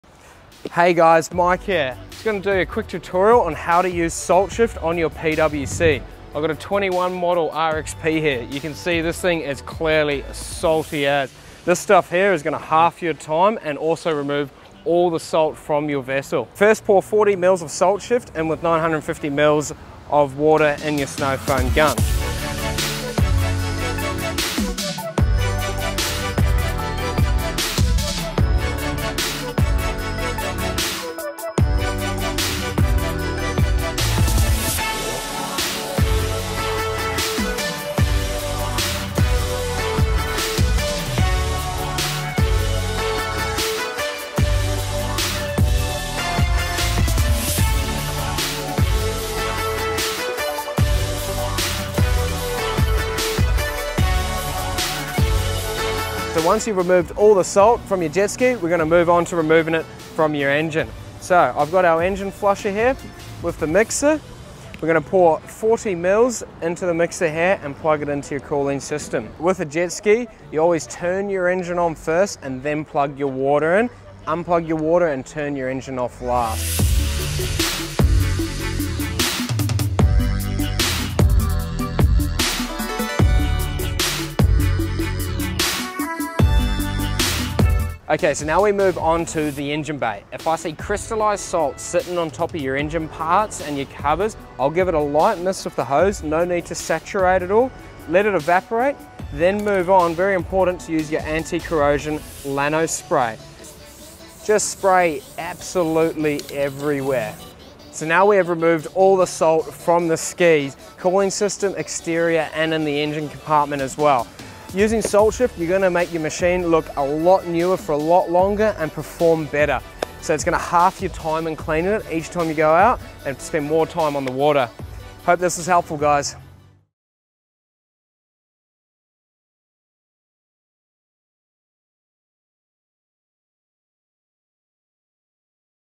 How to Clean your Jet Ski and Engine in 30 Seconds
Over the past 14 months Mike and the crew have had a little something brewing up here at JET TECH with their buddy Paul the Chemist.

Introducing our sister from another mister SALT SHIFT, a comprehensive industry leading, marine wash brand, like everything we do we put our heart and soul into it, to create something new and more advanced than anything else out there.

Our advanced formula will dissolve all the salt off your vessel in under 30 seconds (fast we know) leaving your machine to gleam like it was day one.